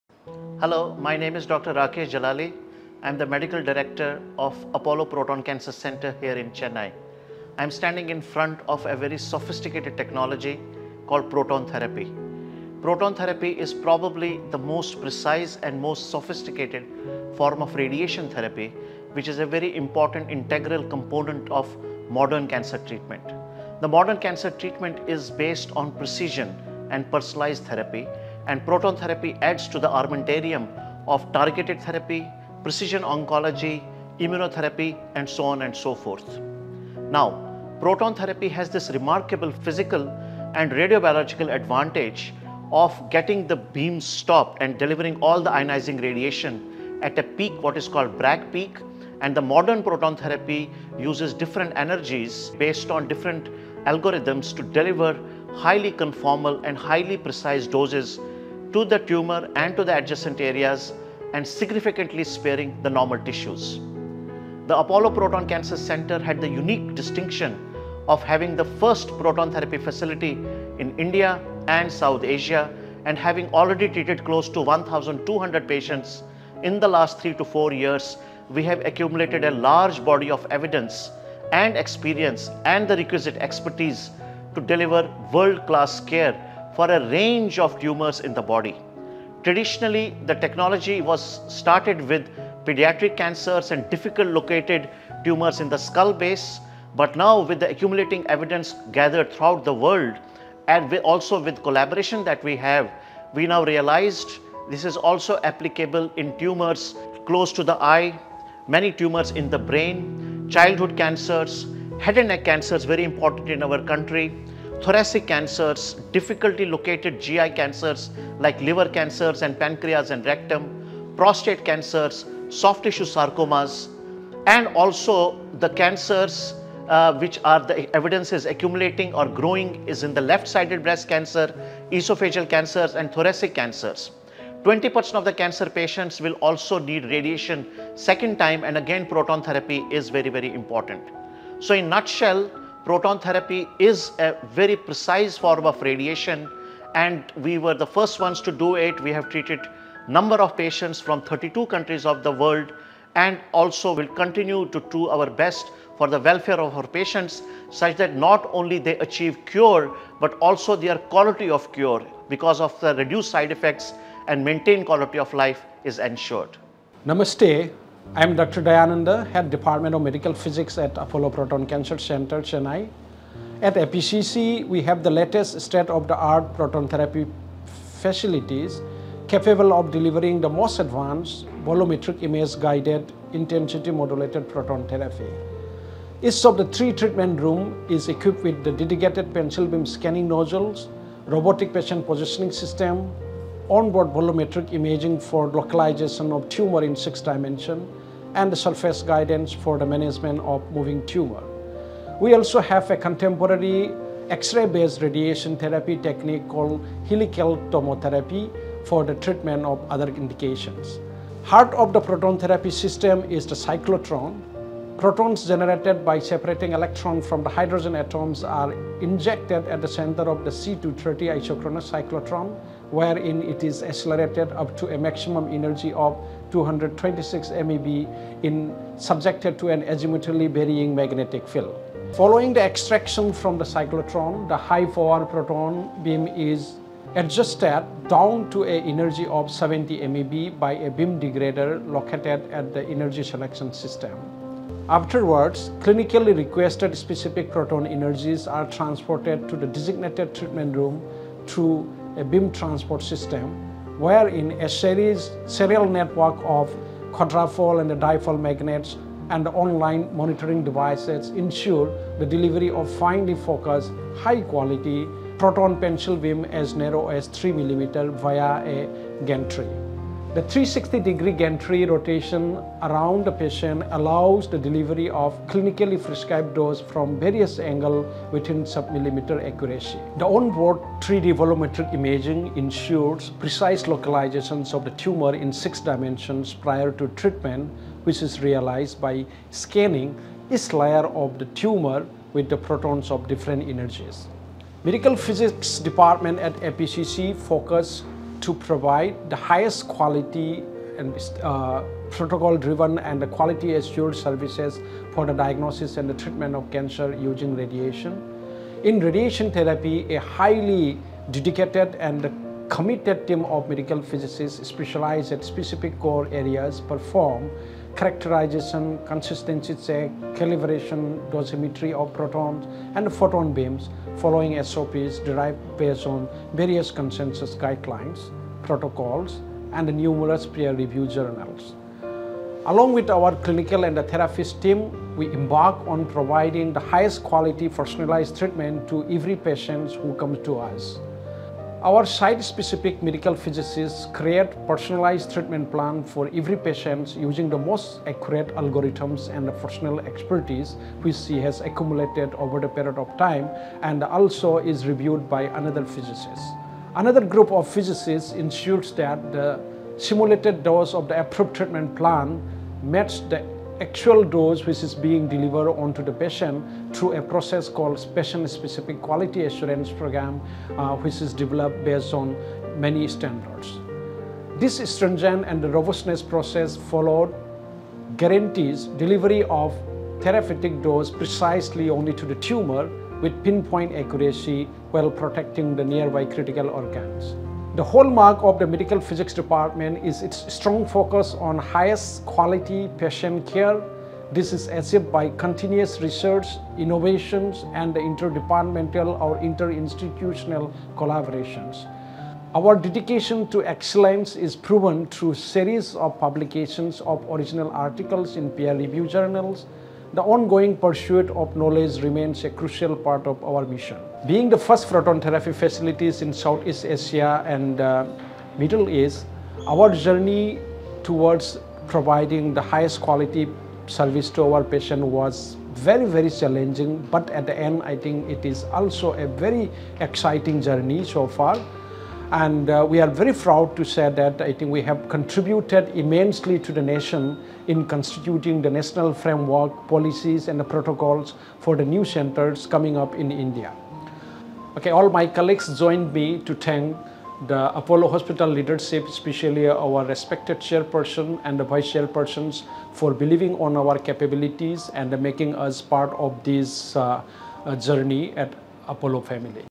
The Expertise Behind Proton Beam Therapy | Apollo Proton Cancer Centre | Chennai
Join Dr. Rakesh Jalali and Dr. Dayananda from Apollo Proton Cancer Centre as they explain how proton therapy is transforming cancer treatment. Learn about the precision, benefits, and success stories of this advanced therapy.